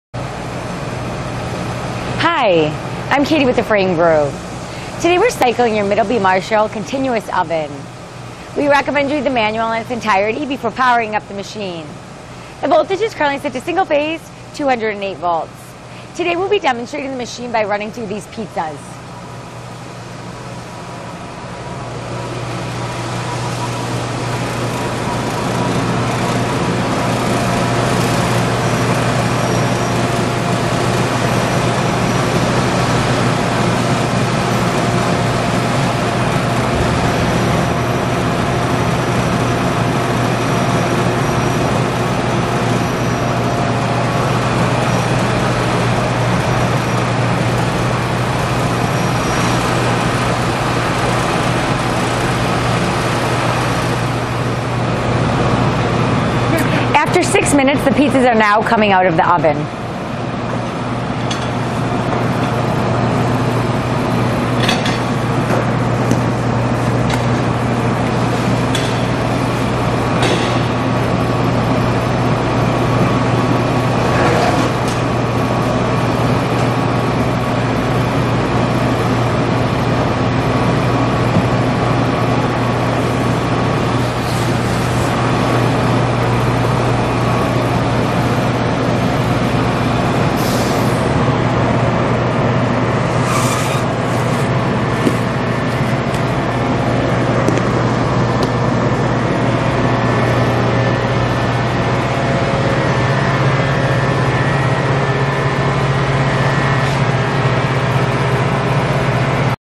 Middleby Marshall Continuous Oven Demonstration
We're demonstrating a Middleby Marshall Continuous Oven!

Single chamber, stainless steel, gas fired, hot air jet oven. Aperture: 32.5" in Width; 4" in Height; 80" in Length. Baking area: 24 ft². Temperature range: 0° - 550° F. Bake time: 1 minute to 30 minutes. Equipped with a 32" wide x 124" long stainless steel mesh conveyor belt, two side viewing ports, 3" diameter exhaust port, two recirculating air blowers, variable temperature/speed controls, and a touch pad controller with LED display. Mounted on height adjustable legs and casters.